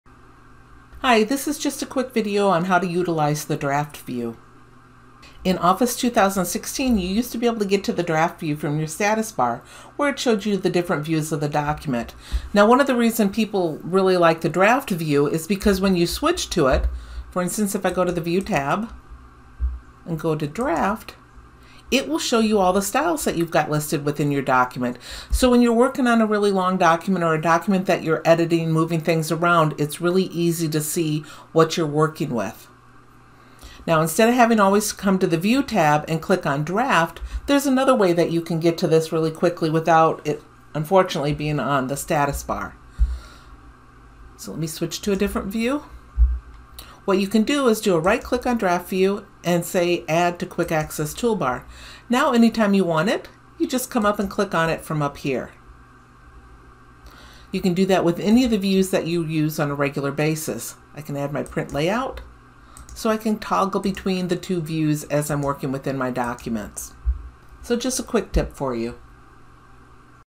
Draft View is Missing from the Word Status Bar
How can I get the Draft View back, just like it was in Word 2016?
In later versions of Word, Microsoft decided to remove the Draft View from the Status Bar. In this video, I will quickly show you how to solve this problem.